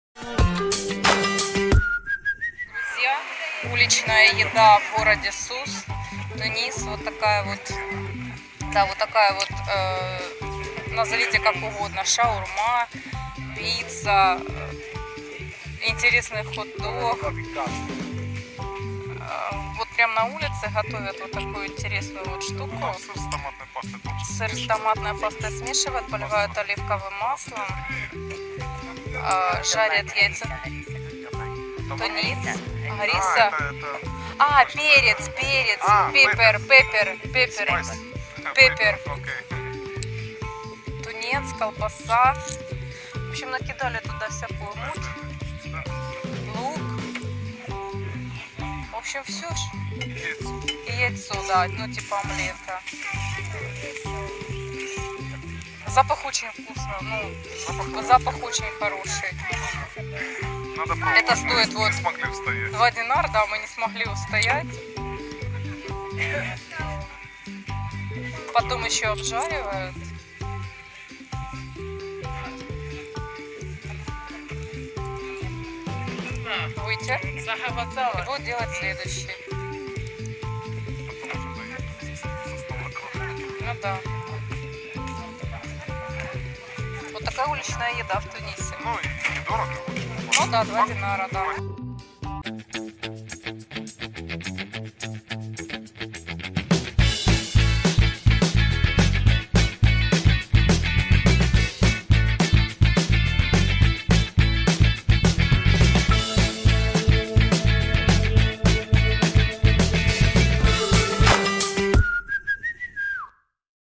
Уличная еда|Что едят в Тунисе| Пробуем чапати,шаурма в Суссе
Уличная еда в Тунисе, опасно ли есть на улицах в Суссе? Мы рискнули и не пожалели. На рынке Медина в Тунисе в городе Сусс, продают очень много уличной еды: пирожков с начинкой, хот- догов, шаверм, пицу, сладостей, и многого другого. Мы же наткнулись на название" CHAPATI" и решили попробовать. Позже, прочитав в интернете обозначение этого слова, так и не поняли что же мы тогда ели, но было необычно вкусно.
Берут нечто похожее на лаваш ( но толстый), жарят омлет с овощами, добавляют острый перец, лук,зелень, колбасу, сыр, тунец и все что есть под рукой. Заворачивают, обжаривают и готово.
Цена 2 динара/штука.